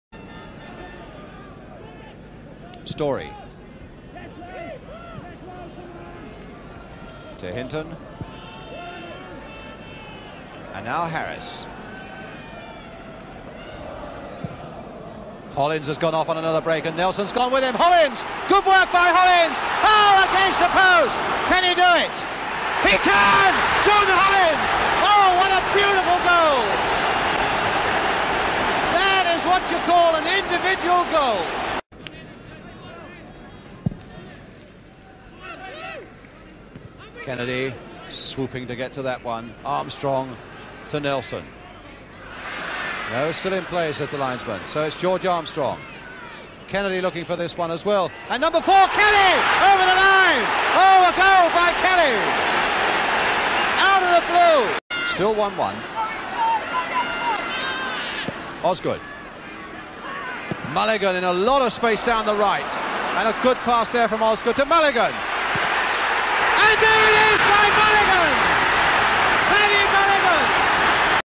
Chelsea 2-1 Arsenal 1970
John Hollins and Paddy Mulligan for Chelsea
Eddie Kelly for Arsenal